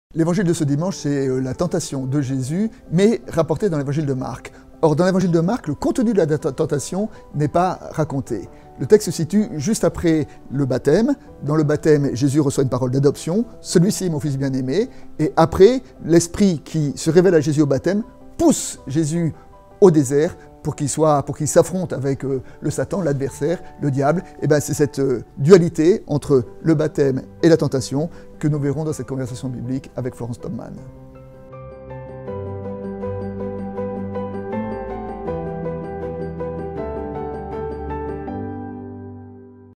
[Teaser] Baptême et mise à l'épreuve de Jésus Marc 1, 12-15 - Antoine Nouis et Florence Taubmann
Les commentaires des pasteurs Antoine Nouis et Florence Taubmann sur le texte biblique de Marc 1, 12-15.